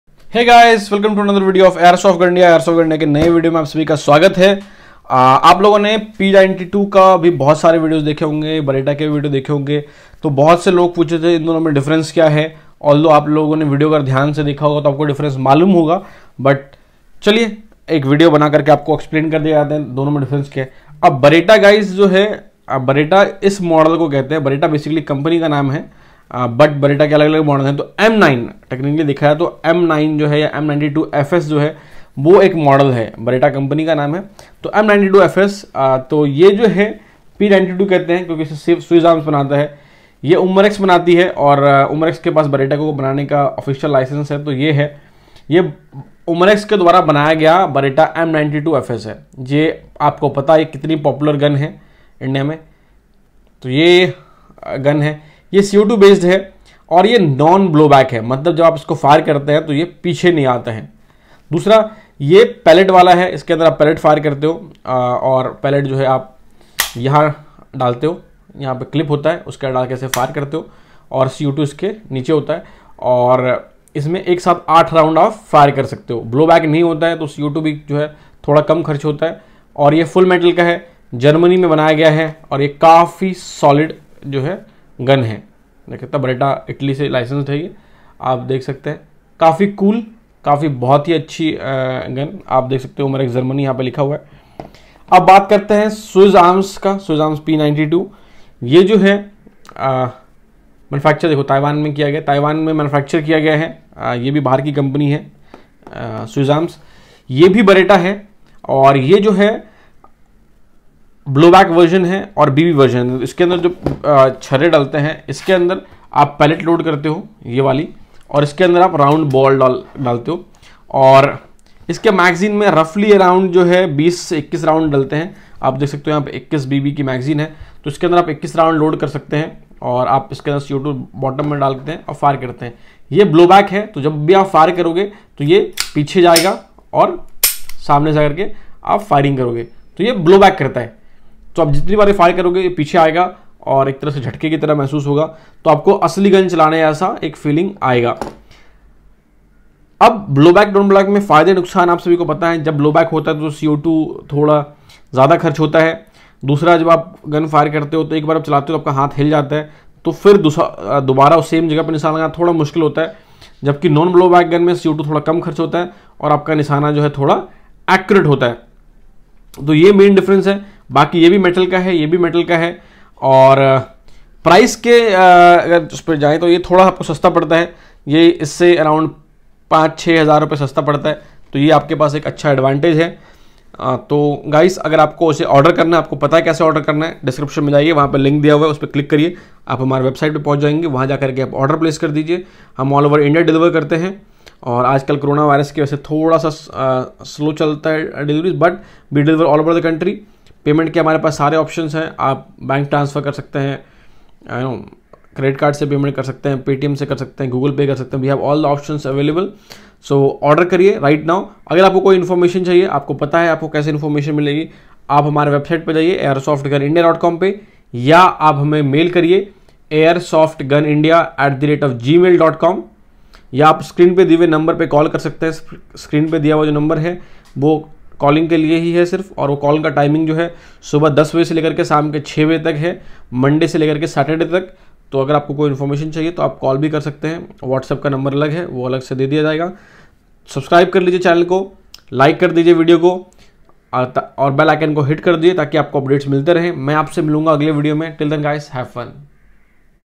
Beretta m92fs co2 Vs Swiss arms Beretta style P 92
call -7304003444
what’s app - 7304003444( Monday to Saturday 10 am to 6pm )
 In this video we will try to explain basic difference between beretta m92fs Vs Swiss arms P-92 Air gun
Choose the model you want wisely

Beretta M9 style Swiss Arms P92 CO2 Pistol
Airsoft gun india present super amazing co2 bb pistol that looks amazing Beretta M9 style Swiss Arms P92 CO2 Pistol
This is a fun gun. It’s accurate, has a reliable mag and feels like a model 92 firearm. Not a lightweight gun, it actually weighs a bit more than the firearm when you’ve loaded it with a CO2 cartridge and BBs.

Beretta FS92
CO2 (uses 12-gram cartridges)
8-rd pellet repeater
Semiauto
Double-/single-action
Blued frame
Black plastic grips
Highly realistic, which means this is as big as the firearm!
Includes a hard case
If you enjoy shooting replica pistols, this is one to add to your collection. Highly realistic, the Beretta FS92 airgun is a CO2 pistol that will help you stay proficient with your firearm…or just give you hours of plinking fun. No matter what reason you have for getting this air pistol, you’re gonna love it!